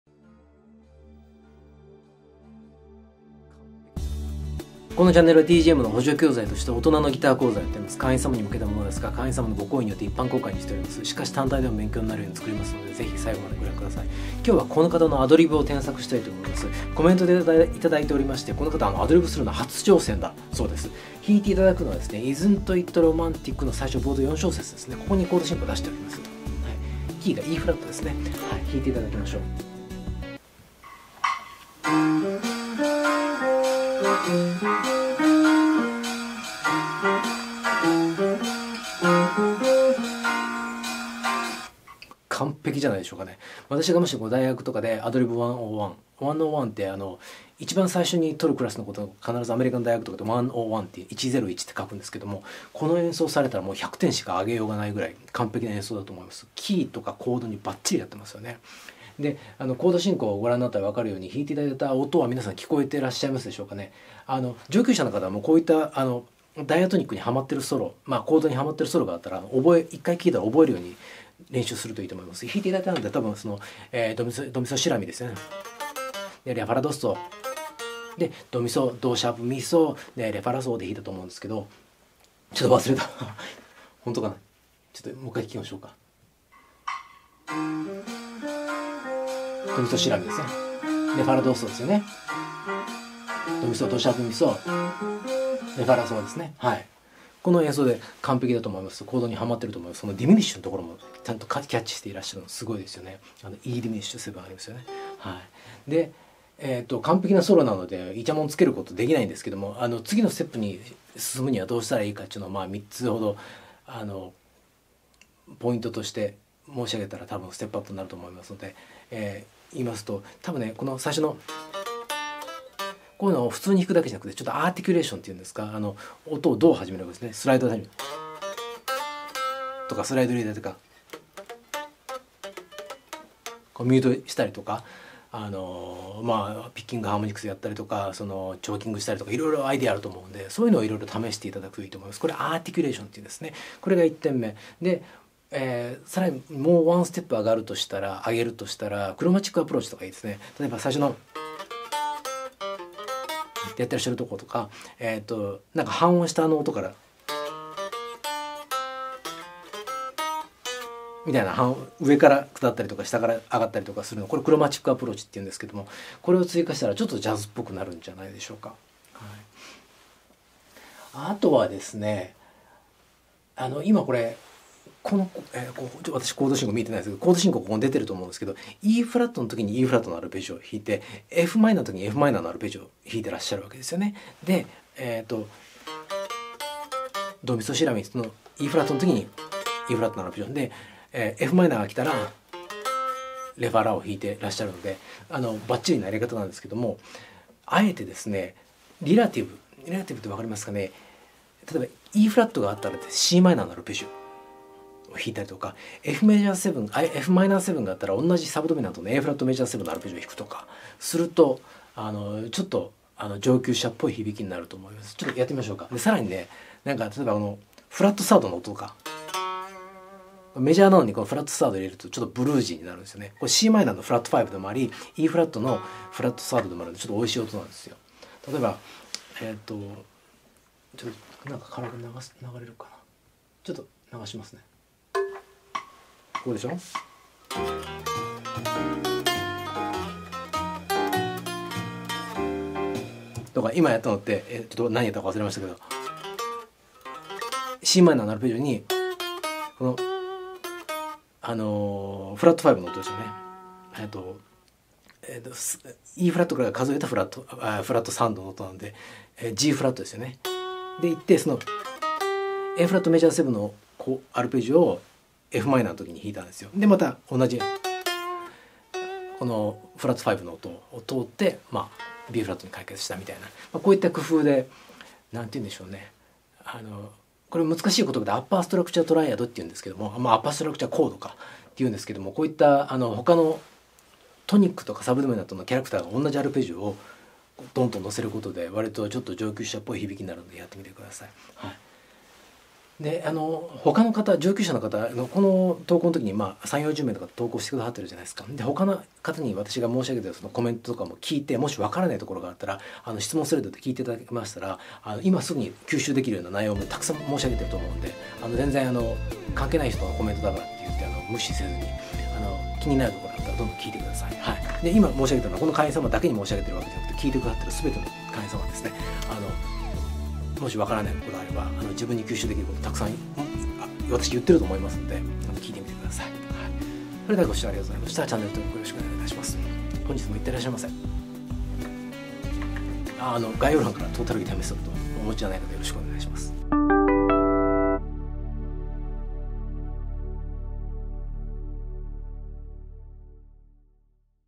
【第29回】はじめてのアドリブ【大人のギター講座】
こんなアドリブからスタートできるギタリストがいるなんて信じられないです。普通はスケールの往復で終わるはずなのに、この方はなんとアルペジオをキレイに使いこなしてます。お見事です。

記憶聴音についてはこちらの記事をご覧下さい。
トータル・ギター・メソッドⓇを使用して記憶聴音を身に付けて下さい。
【ギター教材の集大成：トータル・ギター・メソッドⓇTGM】